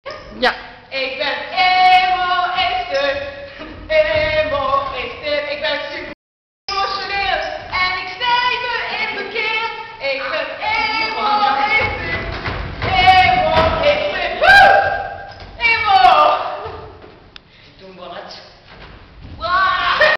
Emooooo Eeeesterr!
Gedaan met Mega Mindy! Hier is Emo Ester! DE NIEUWSTE EN MEEST SEXAY SUPERHERO ALLERTIJDEN!